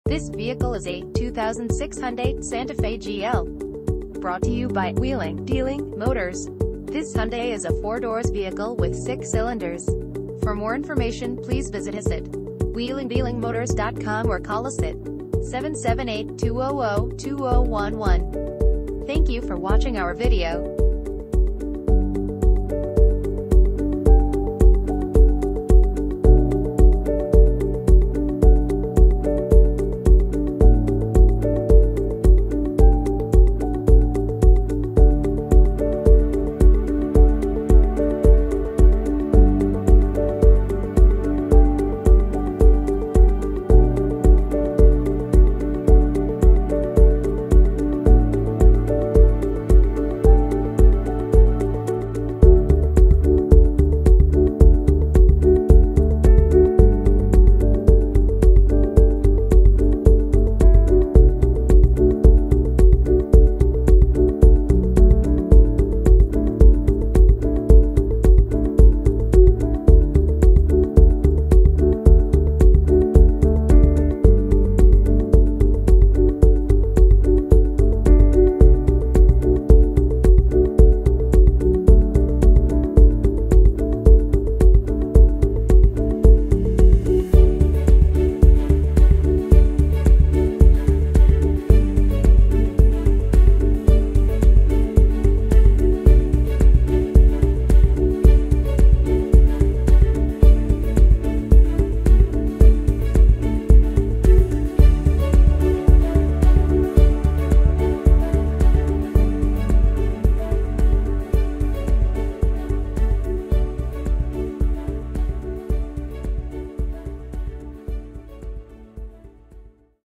2006 Hyundai Santa Fe - Wheeling Dealing Motors
Wheeling Dealing Motors
2540 Bowen Road
Nanaimo, BC V9T3L3

2006 Hyundai  Santa Fe

Clean title, Extended Warranty Available, Automatic, SUV, AWD: All Wheel Drive, 4 doors, 6 cylinders, Gasoline.